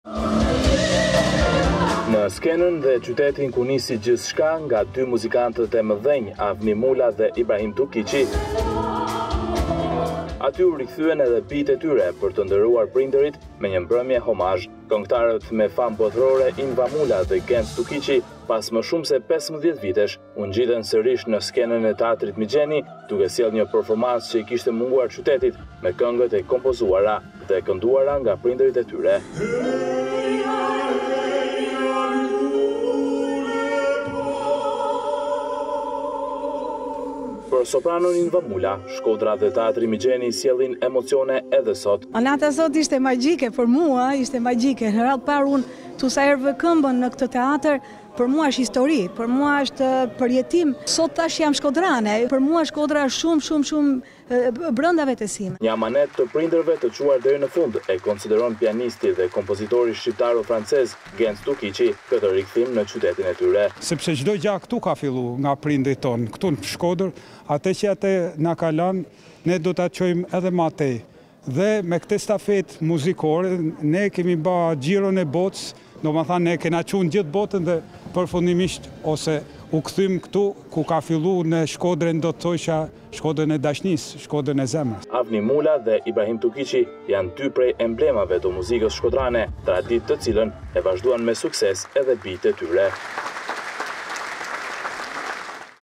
Top Channel/ Inva Mula dhe Genc Tukiqi këndojnë këngët e prindërve
Në skenën dhe qytetin ku nisi gjithcka nga dy muzikantët e medhenj Avni Mula dhe Ibrahim Tukiqi, aty u rikthyen edhe bijtë e tyre për të ndëruar prindërit me një mbrëmje homazh.

Kengetaret me fame botërore Inva Mula dhe Genc Tukiqi,  pas me shume se 15 vitesh, u ngjiten serish në teatrin “Migjeni” duke sjellë një performace që I kishte munguar qytetit, me kenget e kompozuara dhe kenduara nga prinderit e tyre.

Per sopranon Inva Mula, Shkodra dhe teatri “Migjeni” I sjellin emocione edhe sot

Një amanet të prinderve të cuar deri në fund, e konsideron pianisti dhe kompozitor shqiptaro-francez Genc Tukiqi, ketë rikthim në qytetin e tyre

Avni Mula e Ibrahim Tukiqi janë dy prej emblemave të muzikes shkodranë, traditë të cilen e vazhduan me sukses edhe bijte e tyre.